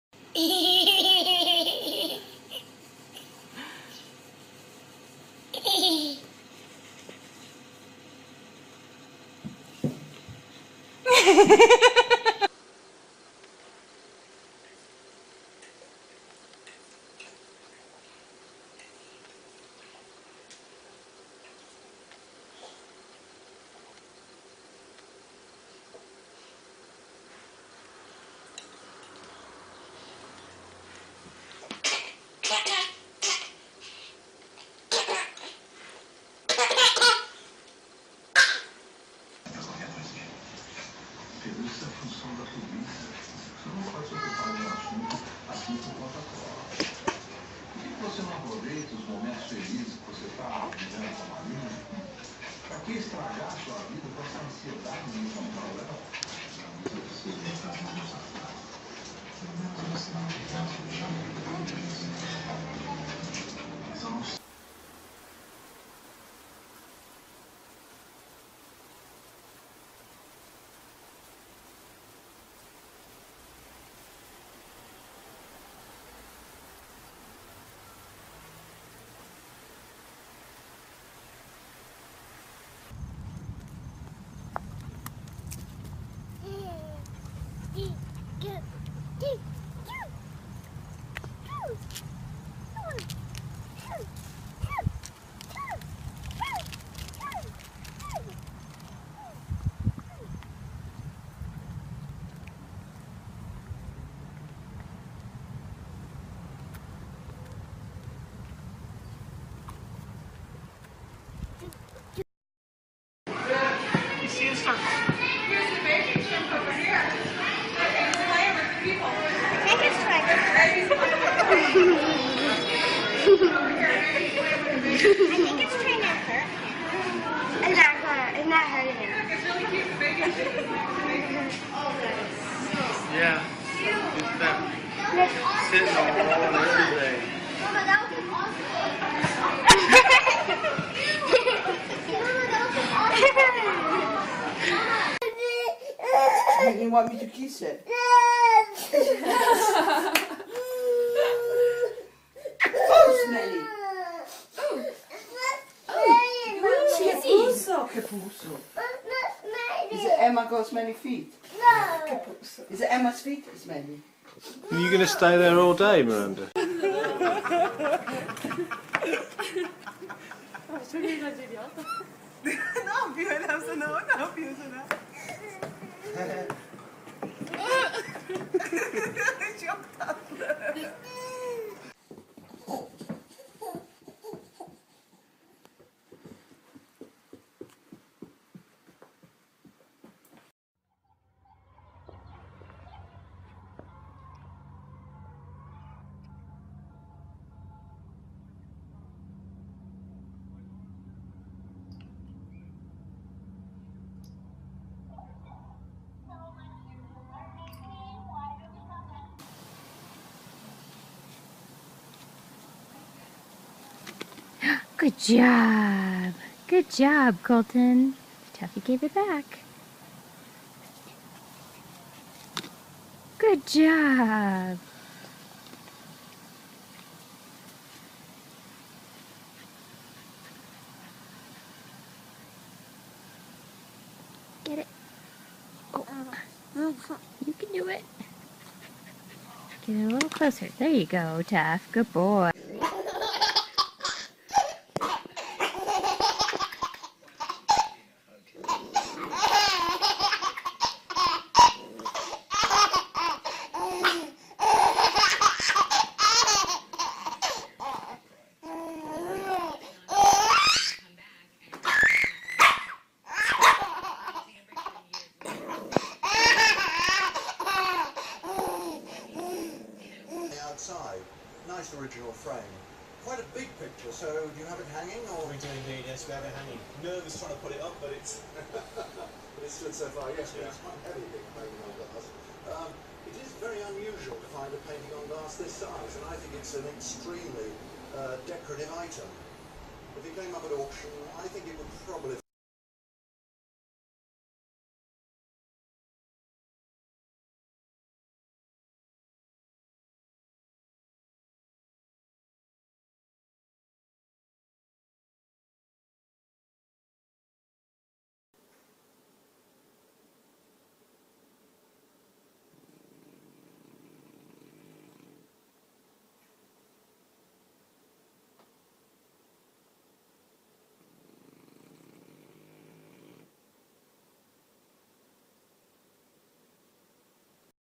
cute dogs and babies playing together compilation 2016
Dogs love their fur-less counterparts, babies.In this cute dog and baby video, tune in to see some precious and hilarious moments between dogs, puppies, babies, and toddlers. Precious.